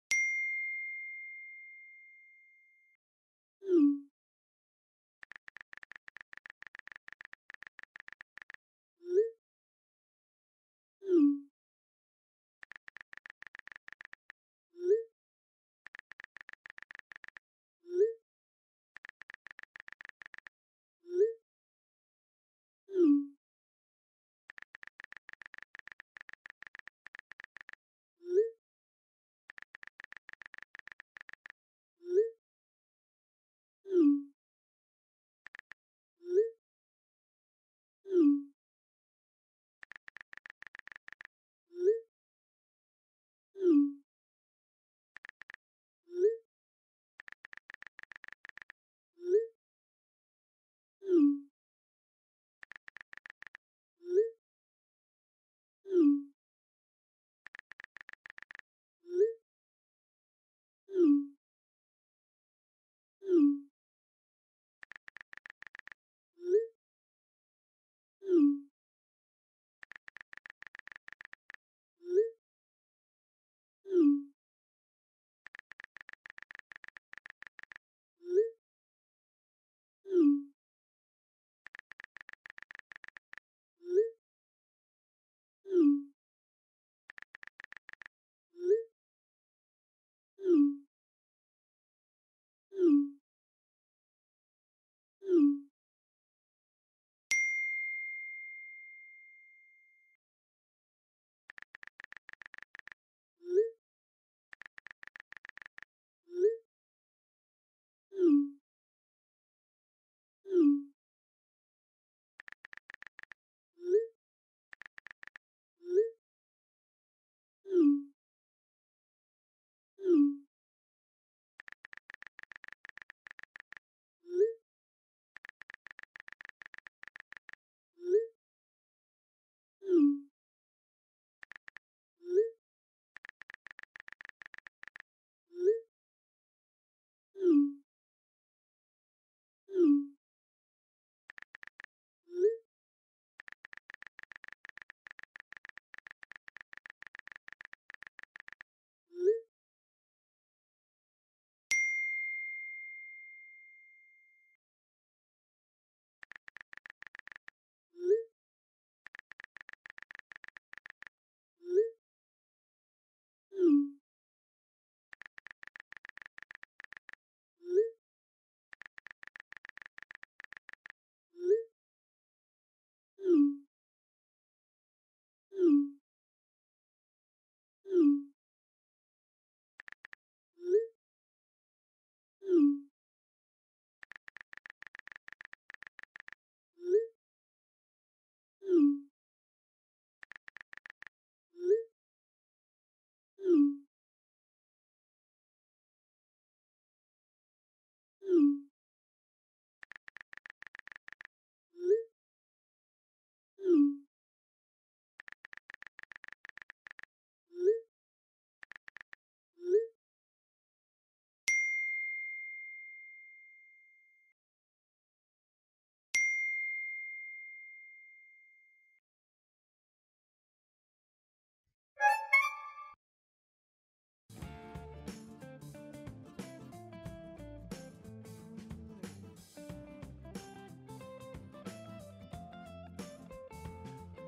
MY WIFE WANTS A BABY
Phony brings you another good dad :)
A Funny Text Message Prank By Phony Texts

A Phony Texts production.
Written and animated by Simon and Jesse, two brothers, two phonies